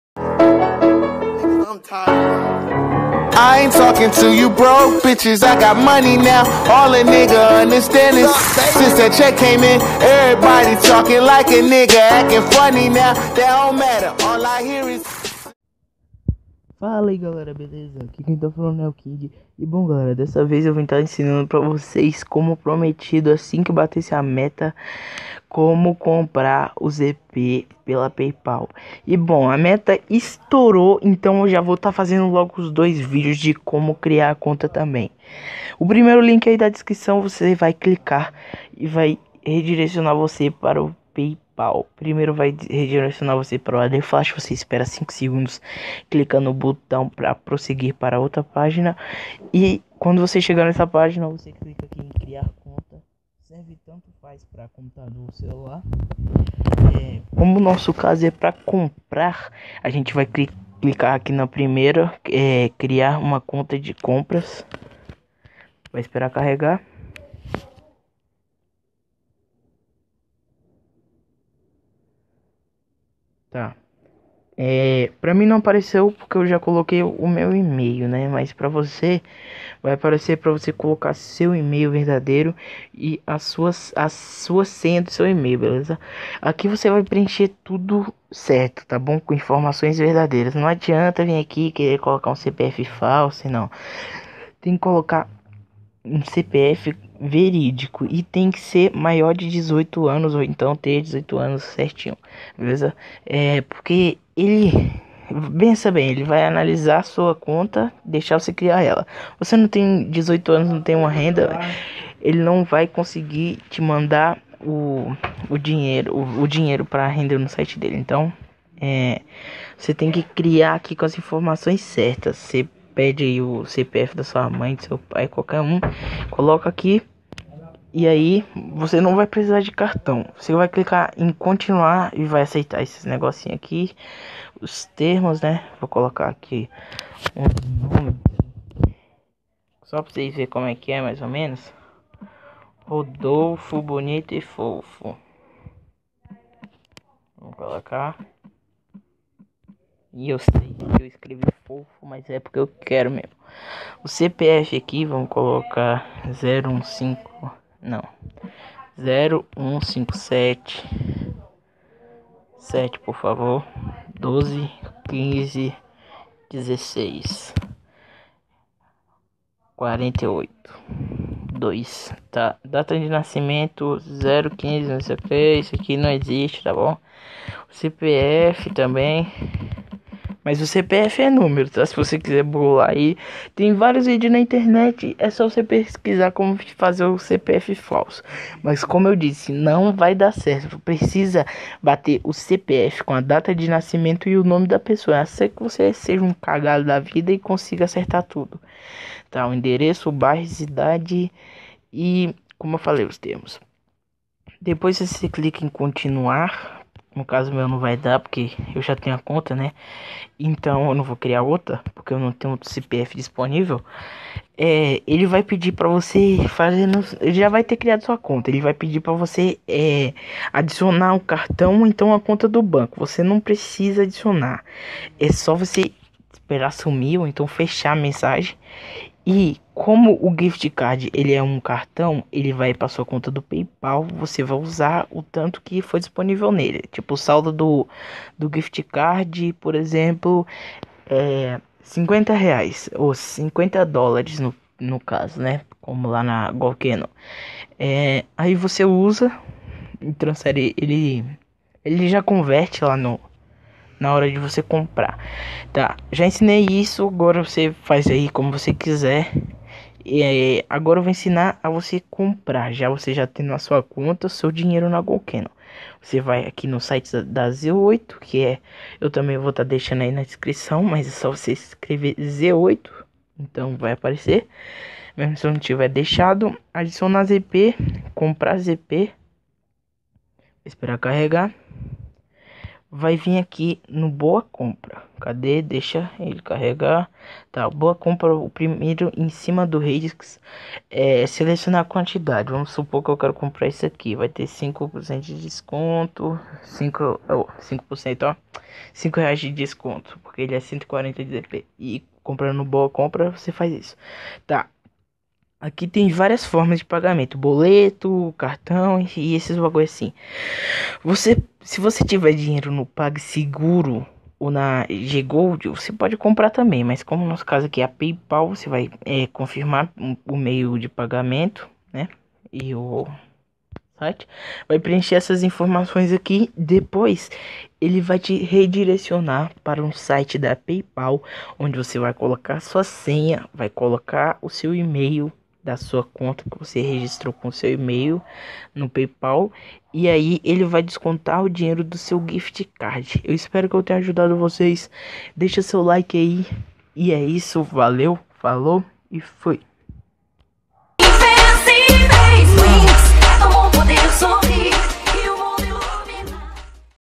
Como Comprar ZP Grátis no CrossFireBR pela PayPal 2018 e criar conta na PayPal sem cartão
ZP rápido e fácil GOKANO vídeos anteriores